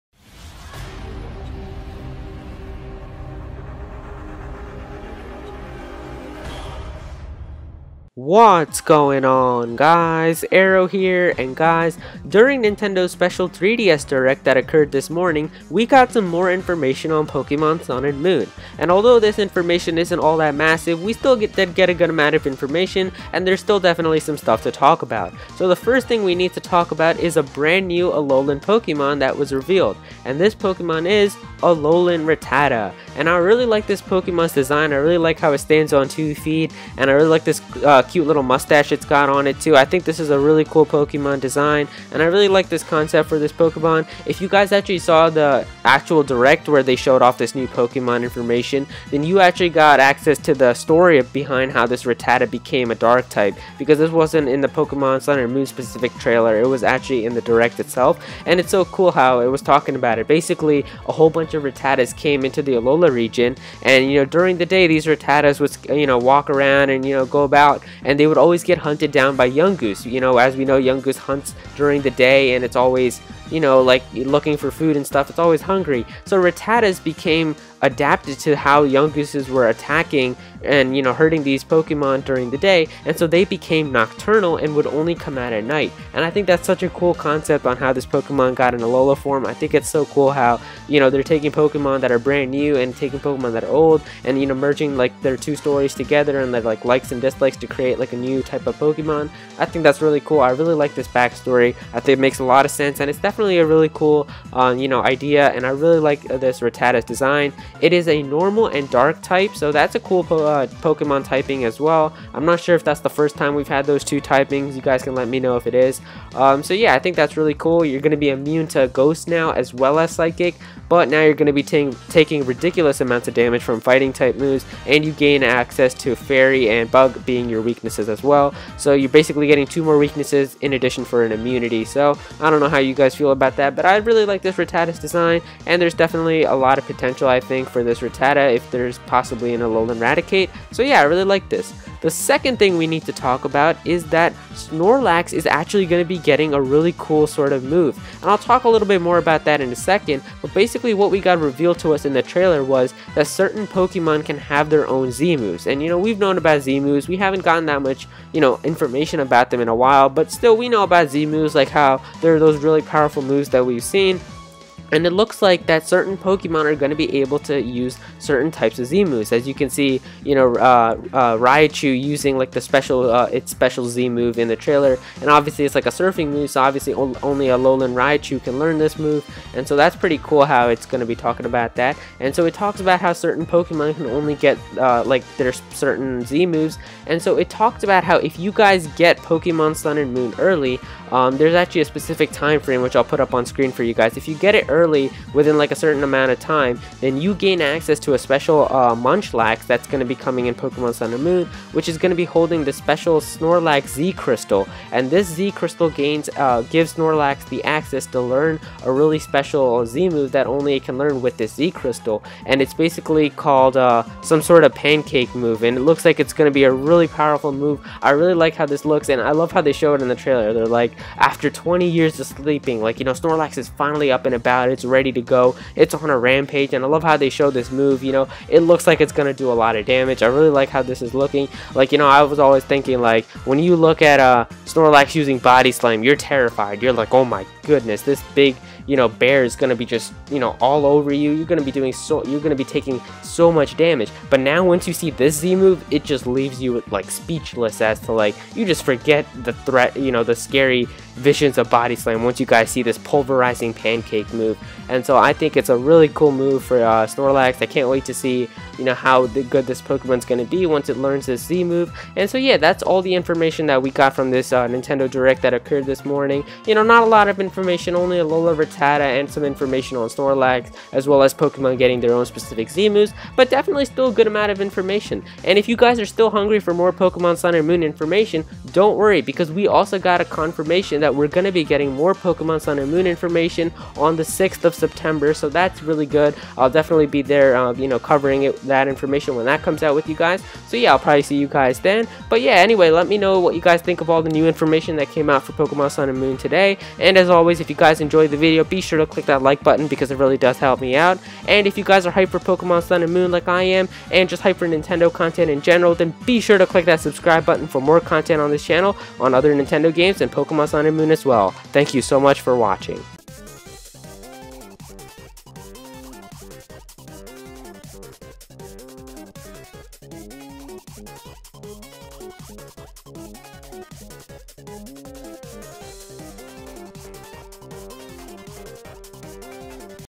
NEW Pokémon Sun & Moon GAMEPLAY and More!
Nintendo's 3DS Direct showed off some new Pokémon Sun and Moon gameplay. I discuss the new gameplay and new information as well as my thoughts on it.